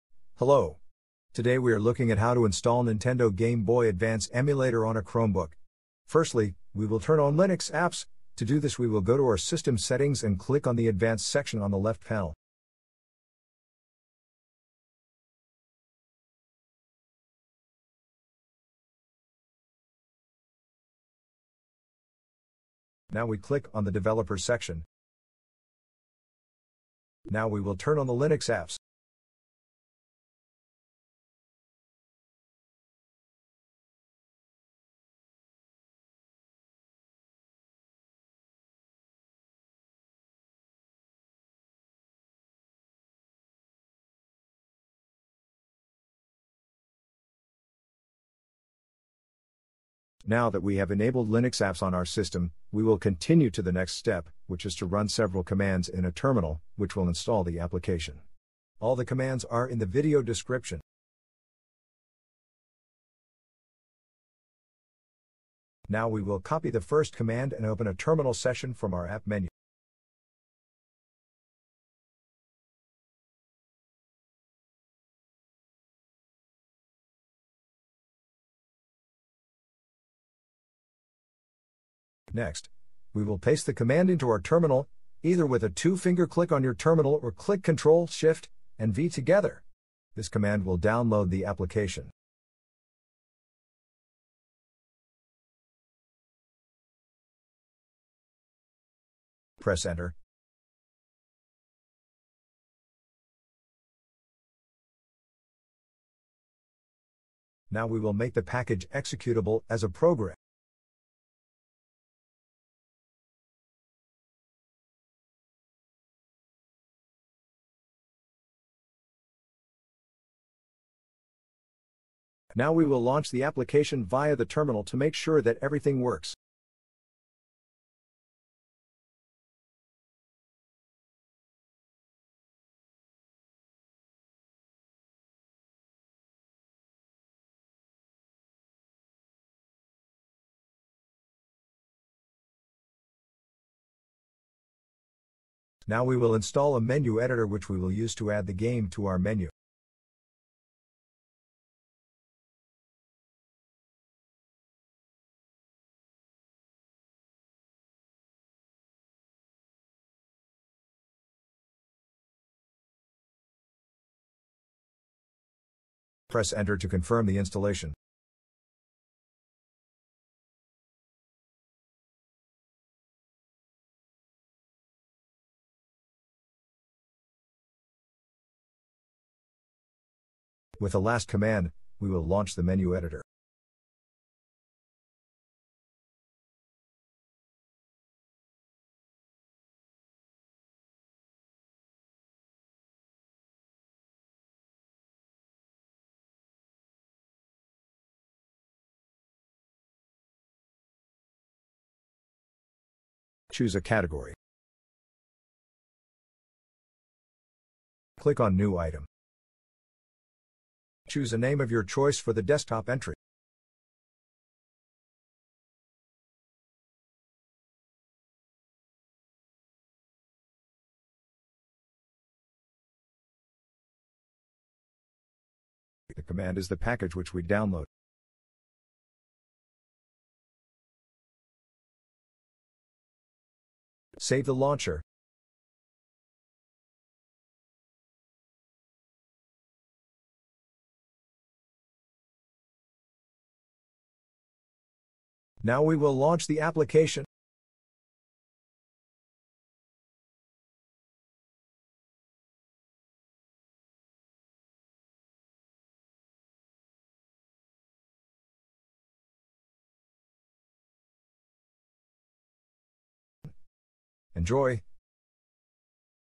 How to install mGBA 0.10.0 on a Chromebook
In this video, we are looking at how to install mGBA 0.10.0 on a Chromebook.